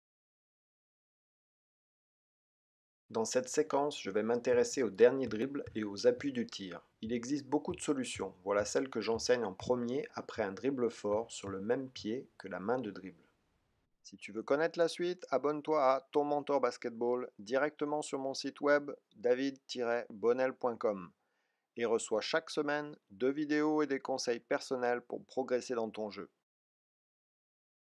Dribble fort basket même pied plus appuis longs alternatifs - Ton Mentor Basketball
Dribble fort basket même pied plus appuis longs alternatifs: Ton Mentor Basketball en savoir plus